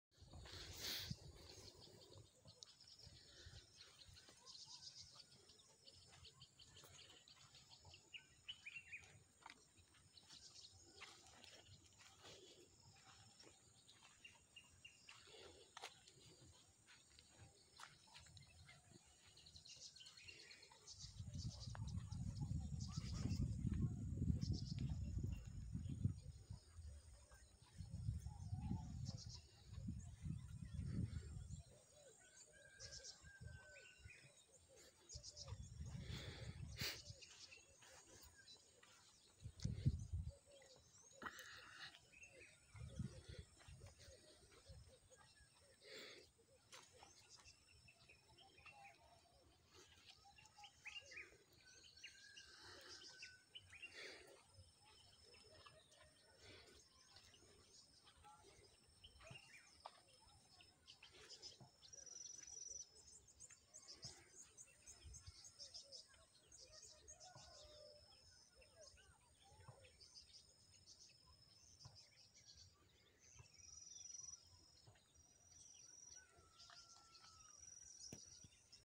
How to make a Passion fruit hanging net support at your farm (part 3)
Starting a passion fruit farm